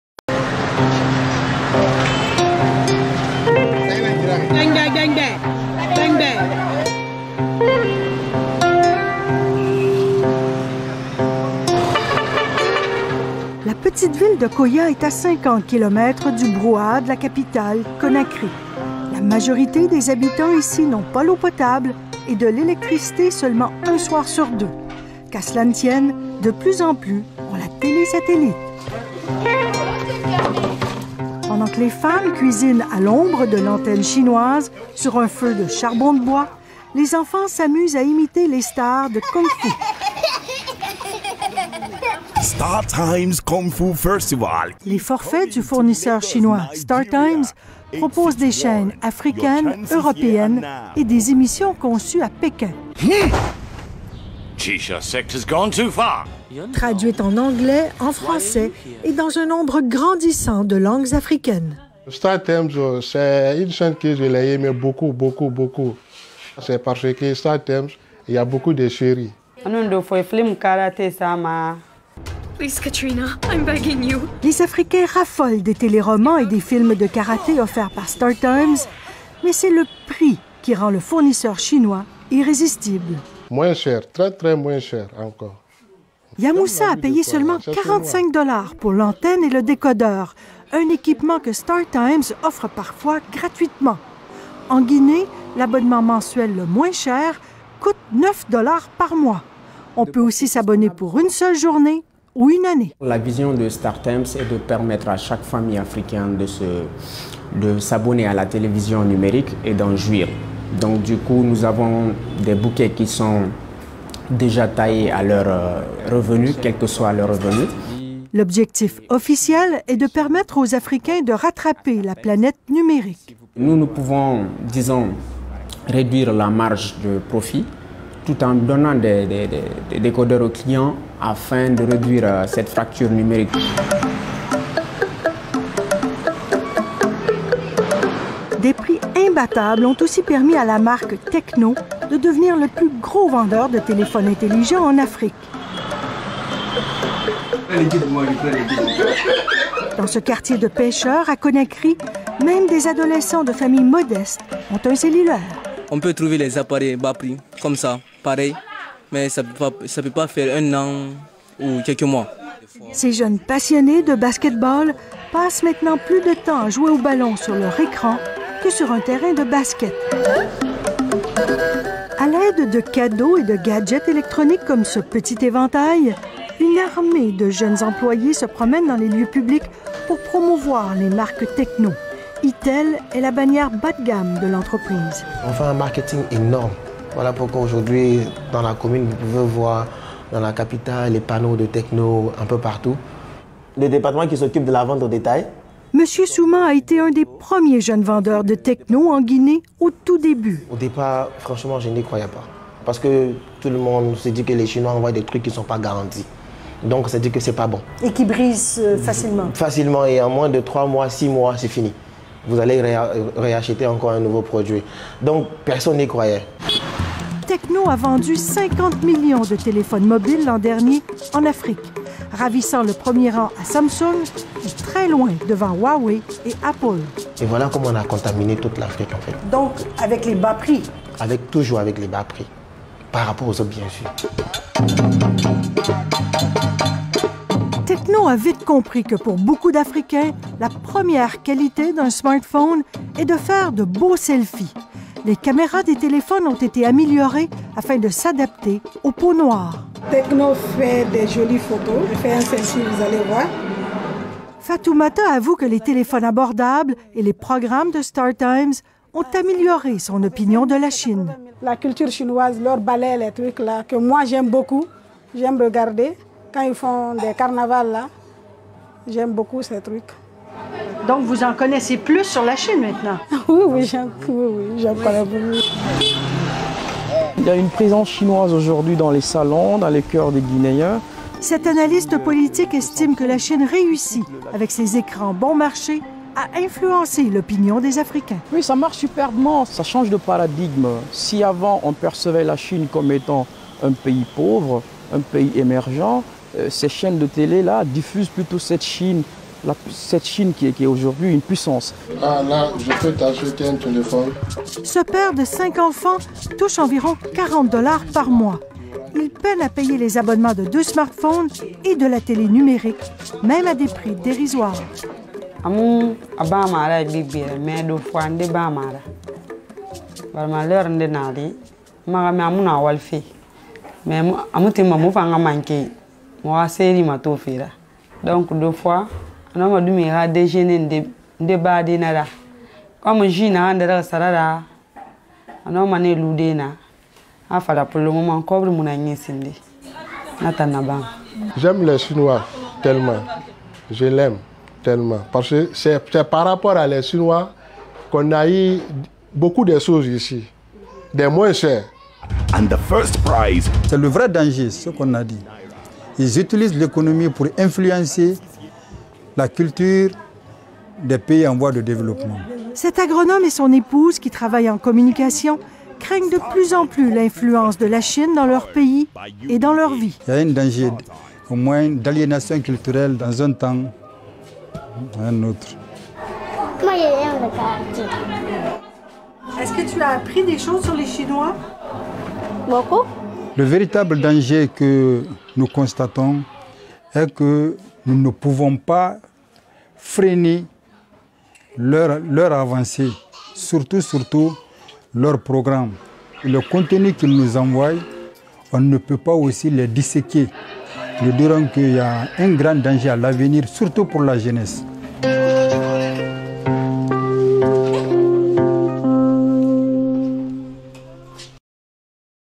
L'empreinte de la Chine🇨🇳 en Afrique notamment en Guinée🇬🇳. 🎥©️Radio Canada Information.
L'empreinte de la Chine🇨🇳 en Afrique notamment en Guinée🇬🇳.

🎥©️Radio Canada Information.